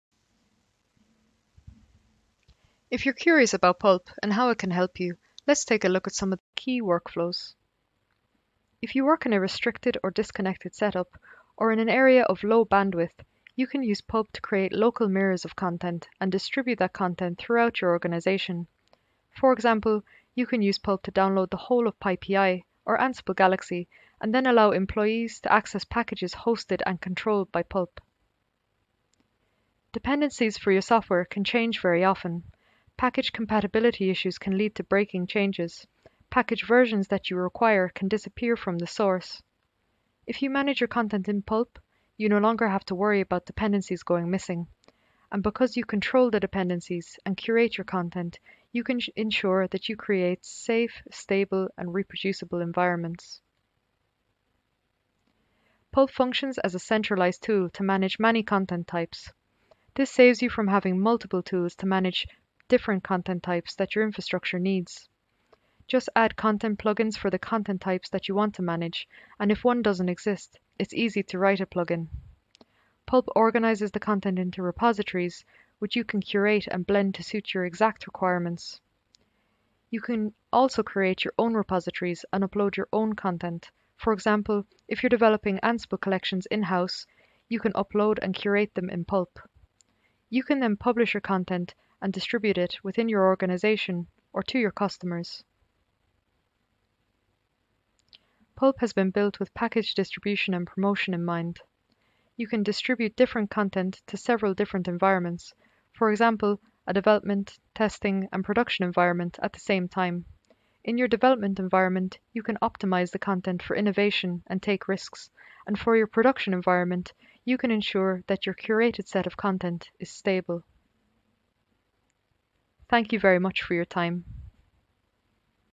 Pulp Workflows
If you want a centralized tool to take full control of your software packages, blend and curate content types to suit your exact requirements, and distribute them throughout your organization, Pulp can help. For a more in-depth look at Pulp's workflows, check out our workflow documentation.
Host your own [PyPI, Ansible Galaxy, container registry, *]

You can use Pulp to create local mirrors of content and distribute that content throughout your organization.
Control dependencies & Create Reproducible Environments

With Pulp, you have full control over dependencies and can curate your content to optimise for your environmental needs.
Multiple Content Types

Pulp has a wide range of available content plugins. Add a plugin for the content types that you want to add. You can also write your very own plugin!

Upload and Manage Your Own Content

You can upload, manage, and distribute your own content.
Content Promotion; Lifecycle Management

You can manage separate sets of content for different lifecycle environments, for example Dev, Staging, Production, and promote content from one environment to another.